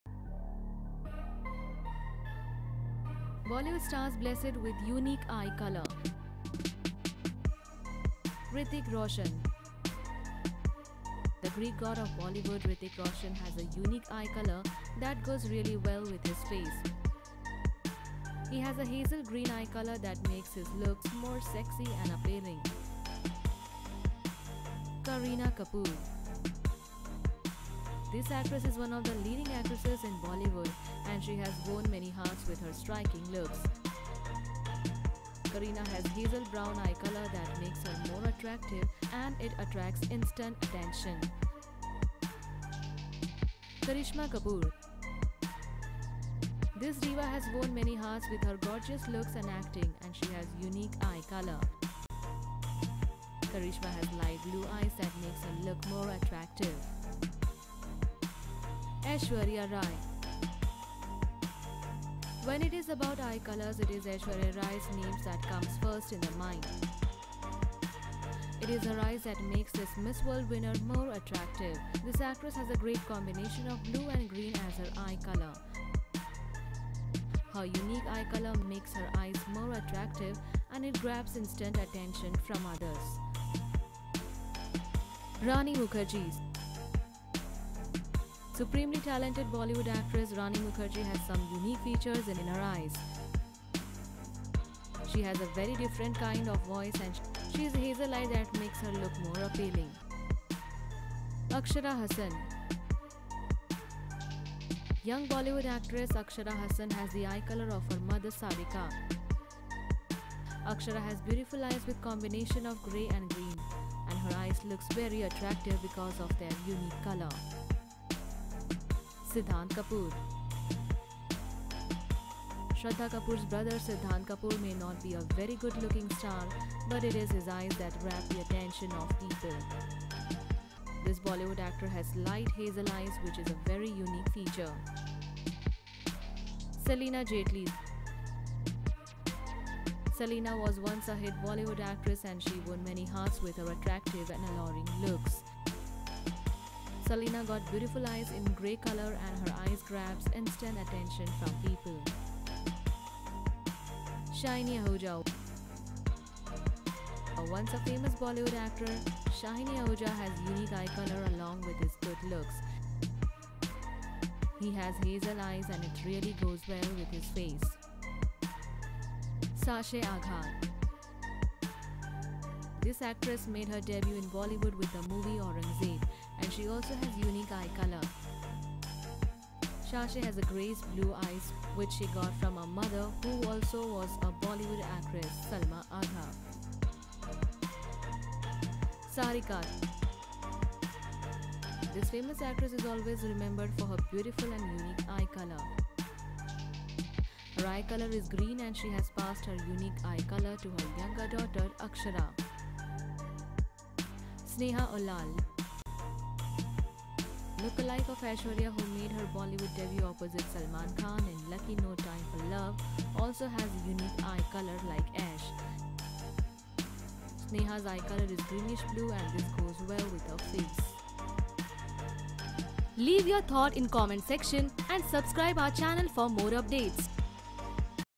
10 Bollywood Stars who Have Amazing Eye Colours
These are the Bollywood Stars who are Blessed With Unique Eye Color :-

1. Aishwarya Rai Bachchan :- When it is about eye colours, it is Aishwarya’s name that comes first in the mind. It is her eyes that make this Miss World winner more attractive. This actress has a great combination of blue and green as her eye colour. Her unique eye colour makes her eyes more attractive and it grabs instant attention from others.

2. Hrithik Roshan :- The Greek God of Bollywood, Hrithik Roshan has an unique eye colour that goes really well with his face. He has a hazel green eye colour that makes his looks more attractive and appealing.

3. Kareena Kapoor :- This actress is one of the leading actresses in bollywood and she has won many hearts with her striking looks. Kareena has hazel brown eyes that make her more attractive and it attracts instant attention.

4. Karisma Kapoor :- This diva has won many hearts with her good looks and acting and she has unique eye colour.  Karisma has light blue eyes that make her look more attractive.

5. Rani Mukherjee :- Supremely talented bollywood actress Rani Mukherjee has some unique features in her. She has a very different kind of voice and she is hazel eyed that makes her more looks more appealing.

6. Akshara Hassan :- Young bollywood actress Akshara Hassan has the eye colour of her mother Sarika. Akshara has beautiful eyes with combination of grey and green and her eyes look very attractive because of their unique colour.

7. Siddhant Kapoor : - Shraddha Kapoor’s brother Siddhanth Kapoor may not be a very good looking star but it is his eyes that grab the attention of people. This bollywood actor has light hazel eyes which is a very unique feature.

8. Celina Jaitley :- Celina was once a hit bollywood actress and she won many hearts with her attractive and alluring looks. Celina got beautiful eyes in grey colour and her eyes grab instant attraction from people.

9. Shiney Ahuja :- Once a famous bollywood actor, Shiney Ahuja has unique eye colour along with his good looks. He has hazel eyes and it really goes well with his face.

10. Sasheh Agha :- This actress made her debut in bollywood with the movie Aurangzeb and she also has unique eye colour. Sasheh has has grayish blue eyes which she got from her mother who also was a bollywood actress, Salma Agha.

11. Sarika :- This famous actress is always remembered for her beautiful and unique eye colour. Her eye colour is green and she has passed her unique eye colour to her younger daughter Akshara.

12. Sneha Ullal :- Look alike of Aishwarya who made her bollywood debut opposite Salman Khan in Lucky – No Time For Love, also has unique eye colour like Aish. Sneha’s eye colour is greenish blue and this goes well with her face.

Reporters : Ria Saha || Aparajita Das